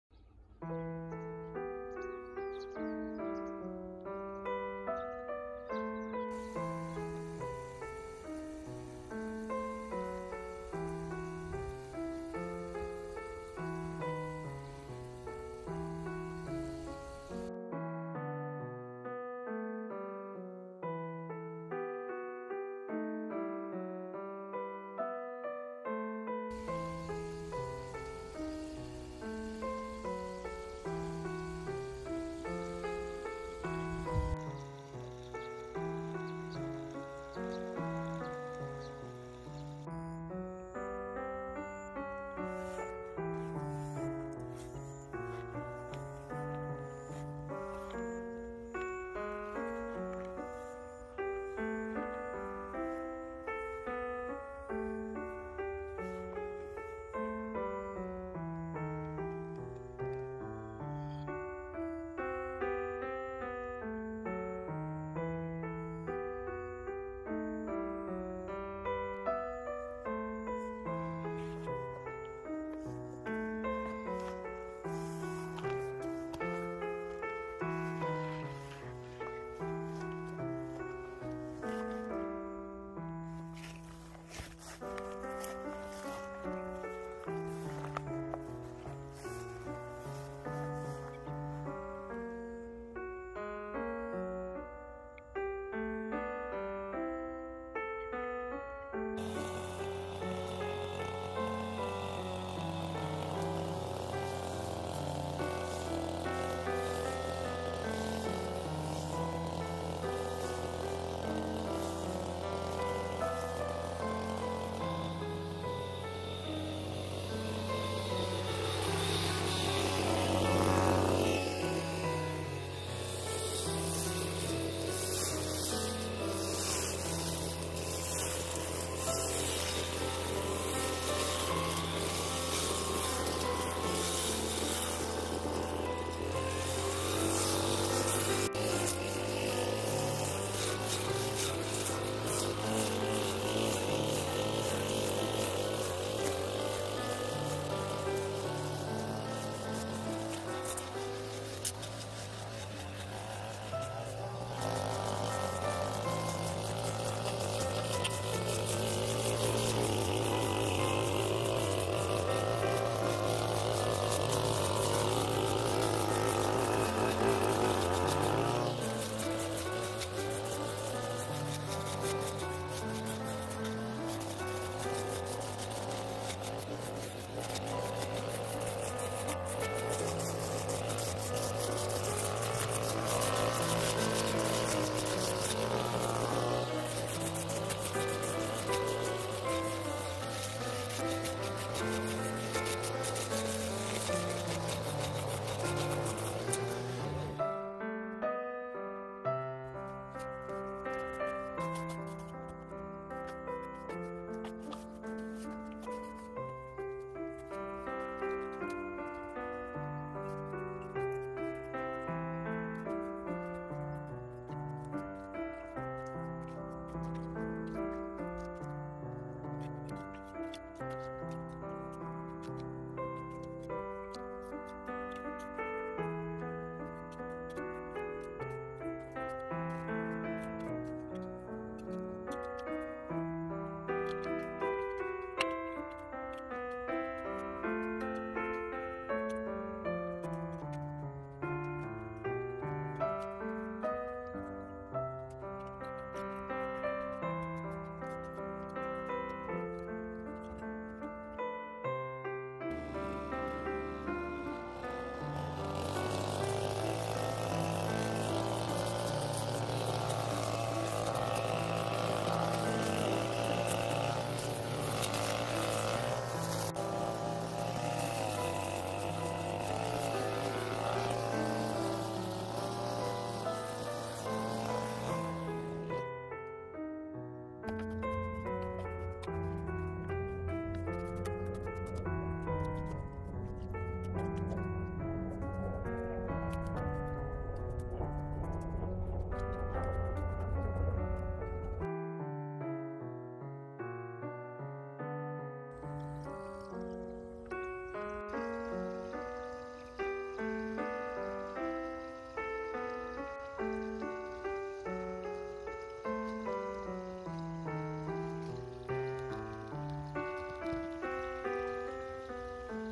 여름철 미나리꽝 관리
무성하게 우거진 미나리꽝
깨끗하게 정리했습니다.